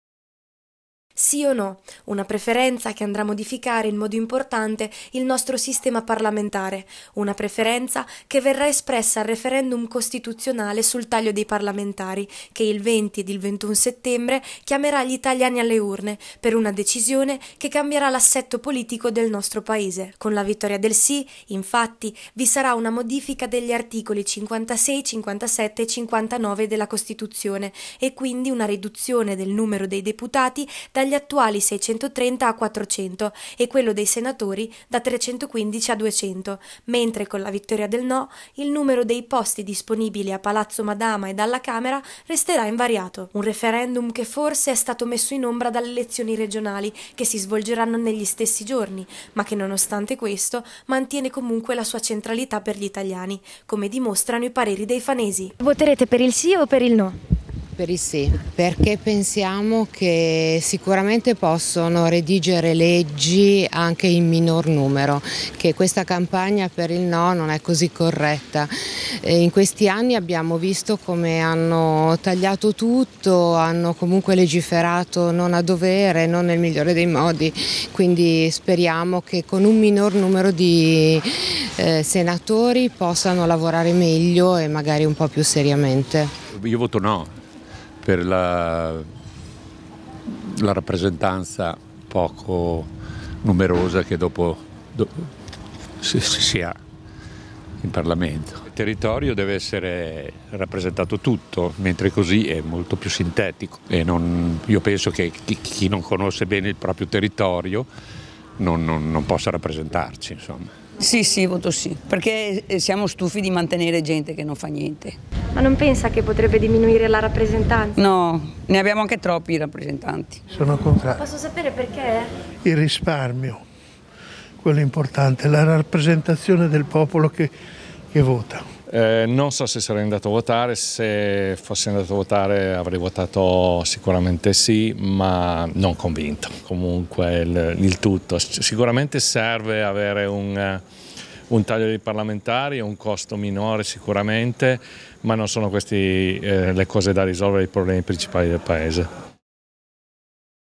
Taglio parlamentari, si o no: i pareri dei fanesi
A breve, in concomitanza con le elezioni regionali, gli italiani saranno chiamati alle urne con il referendum del 20 e 21 settembre, per esprimersi sulla modifica degli articoli 56, 57 e 59 della Costituzione, sul taglio dei parlamentari. Oggi, abbiamo chiesto il parere dei fanesi a riguardo.